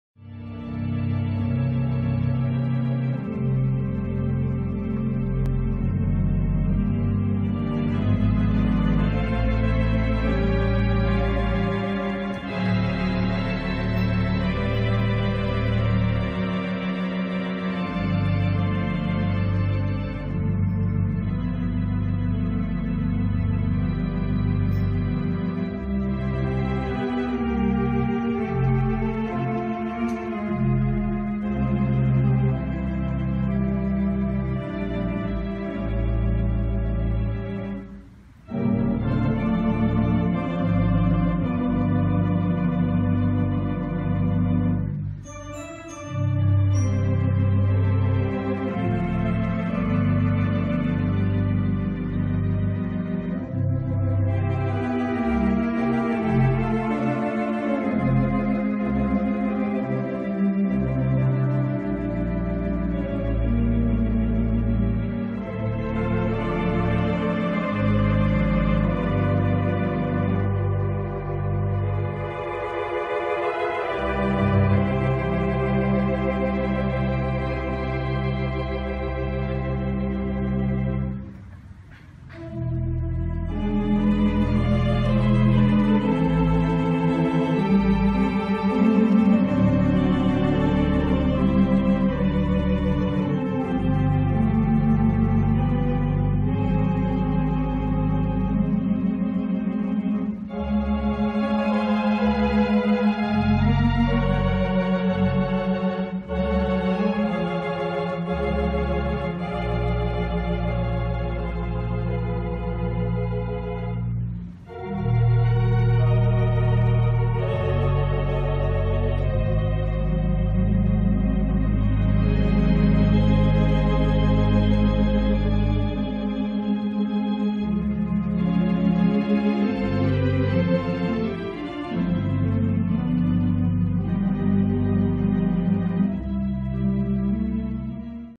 Allen Organ, Model L 332, Wurlitzer Theatre Organ Suite
A demonstration of the Wurlitzer voicing suite on the Allen Organ in Calvary By The Sea Lutheran Church in Honolulu. Dan Dirksen is the organist. This installation was done by Church Organs Hawaii in December of 2015.